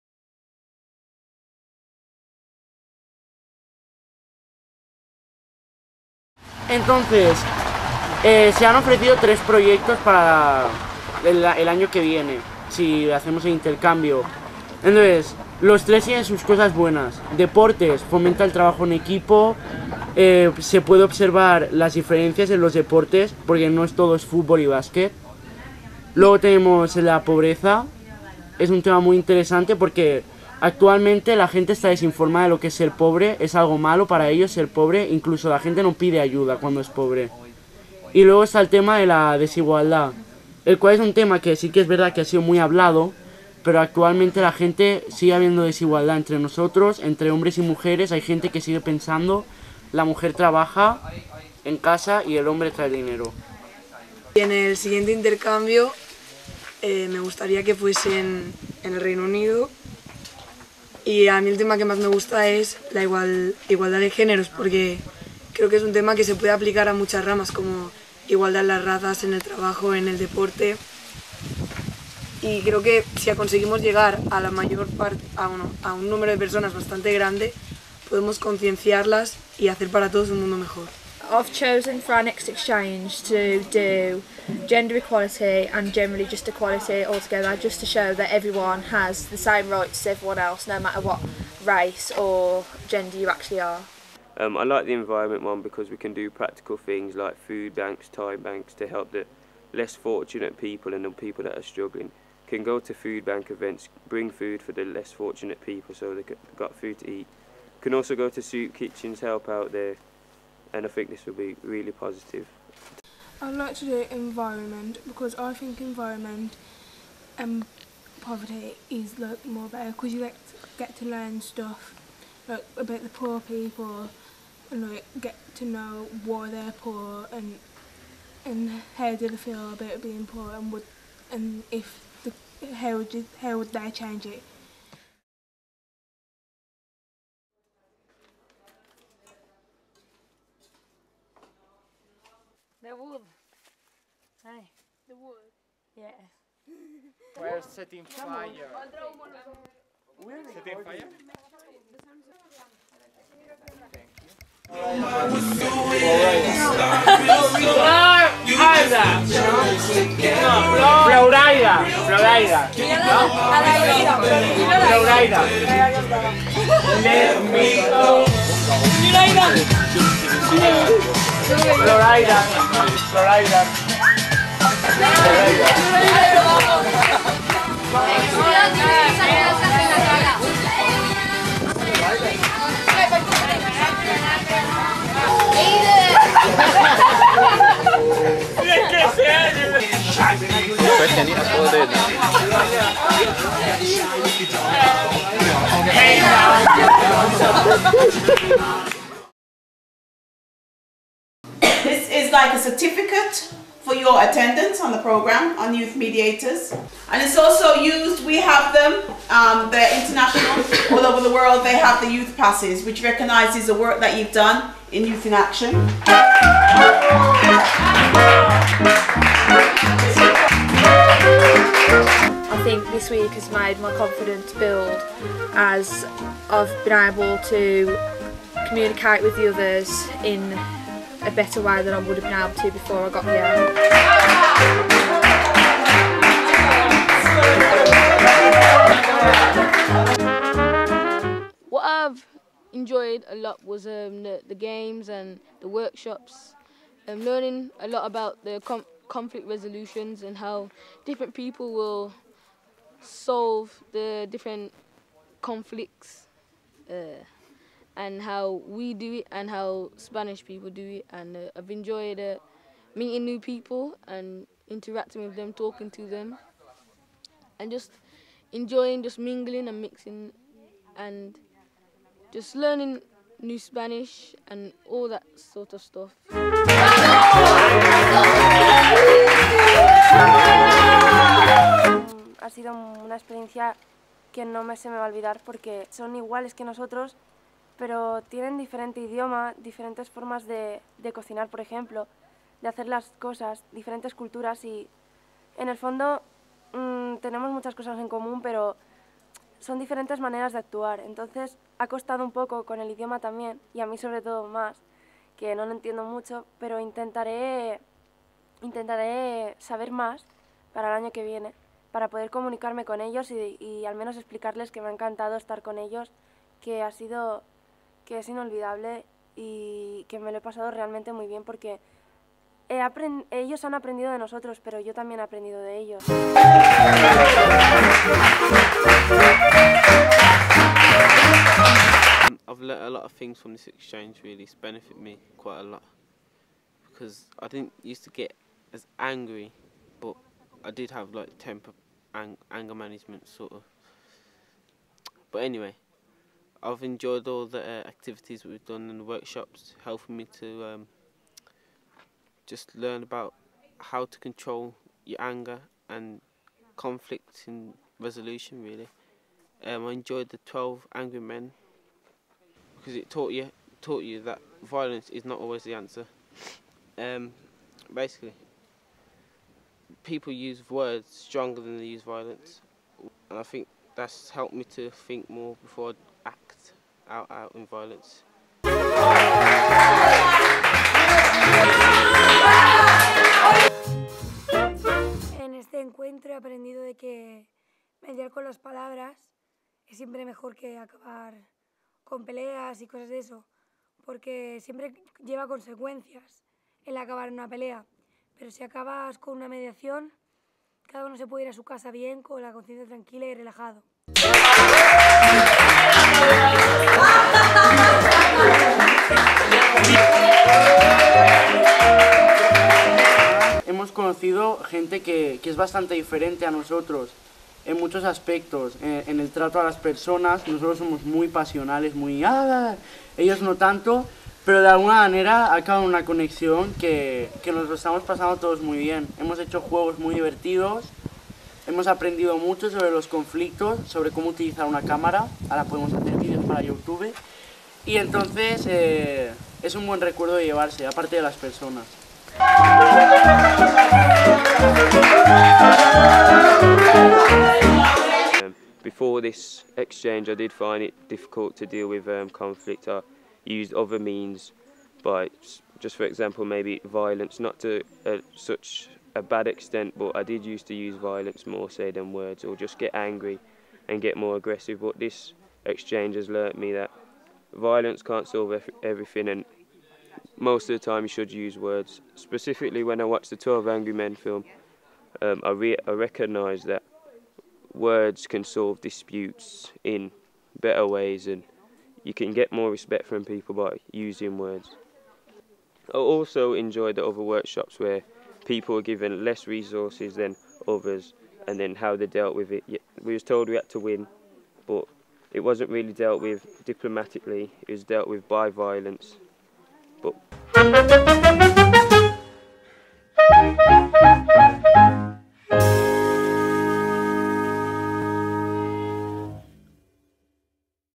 Intercanvi Juvenil Mediadors Juvenils part3
Part 3. Intercanvi Juvenil - Mediadors Juvenils- Vídeo gravat pels participants d'aquest intercanvi que va tenir lloc des del 26 d'agost fins al 2 de setembre de 2012 a Sant Quirze de Besore.
L'intercanvi va ser organitzat per Fora de Quadre com a part del programa Intecanvis Juvenils de Joventut en Acció.